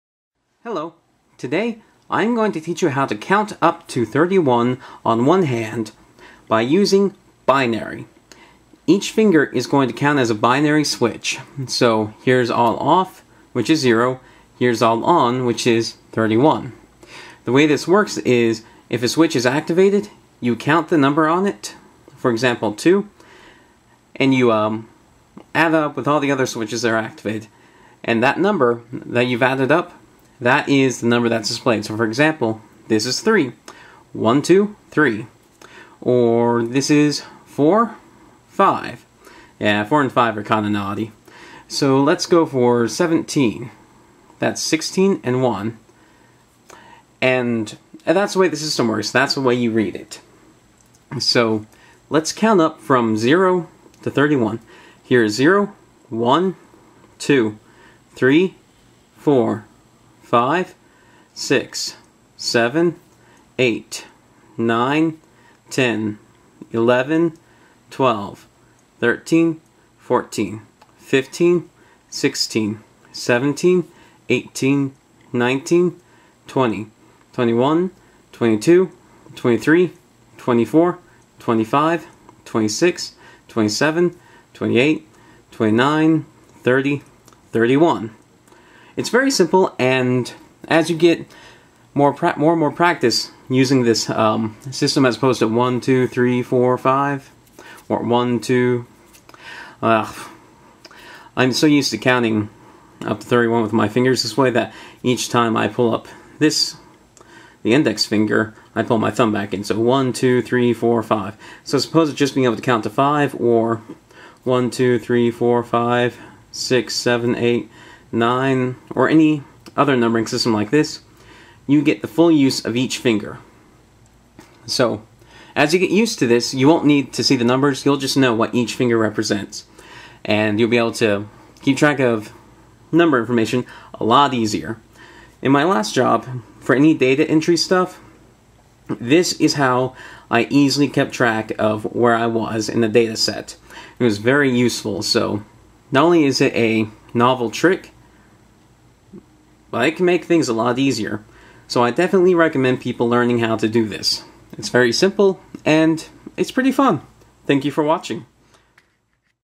How to Count to 31 on One Hand by Using the Binary Counting System
This technique is very simple and easy to learn and it expands the hand's ability to keep track of numeric data. This is also a good way to understand how computers keep track of numeric data because they use this same concept, but on a much larger scale.